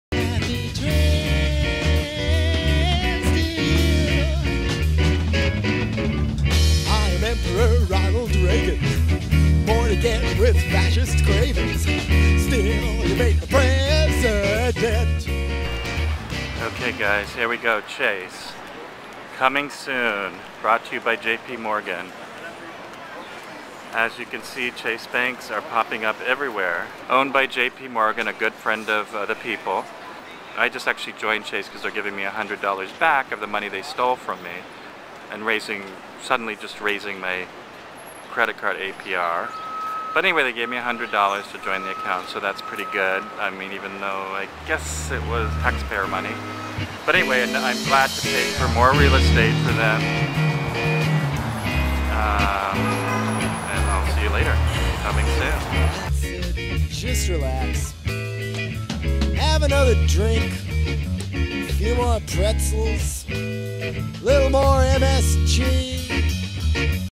FLIPPED OUT MAY 17 2009
Just a little ranting.  23% interest from 8% on credit card and a free 100 dollars free money for opening an account!  Feed me on one end and F%^& me on the other JP!